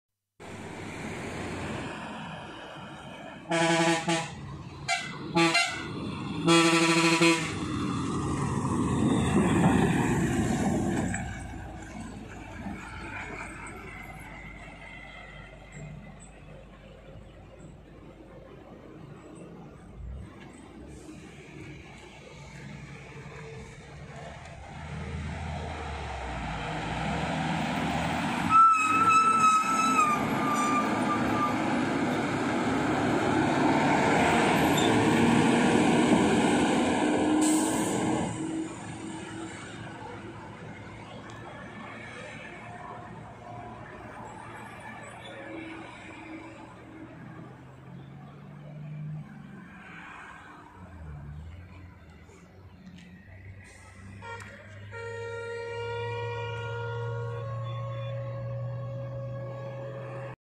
SugarCane Load Lorry & Empty Lorry Turning 27/27 Dhimbam Hairpin bend Driving
SugarCane Load Lorry & Empty Lorry Turning 27/27 Dhimbam Hairpin bend Driving Tamilnadu